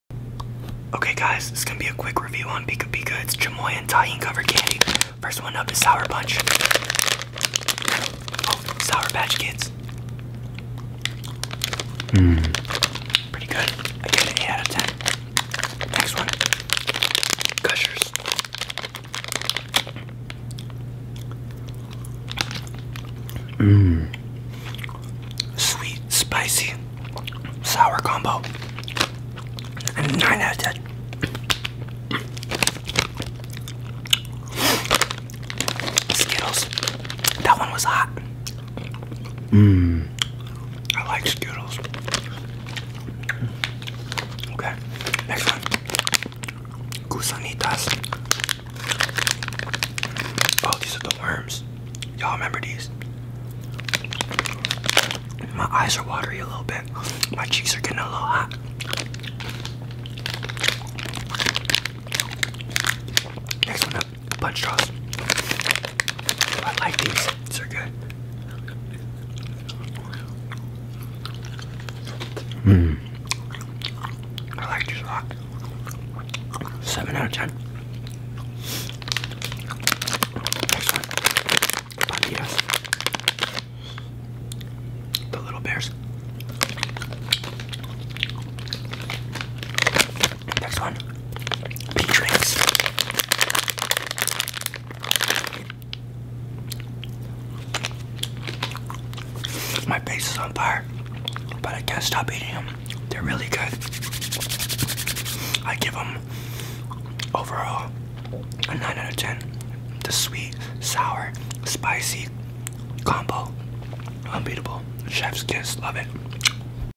ASMR 2 minute FAST paced Pica-Pica review - chamoy and chili covered candies
quick lil breakdown on these awesome little chamoy and chili powder covered candies. Id say probably gushers or skittle was my favorite hbu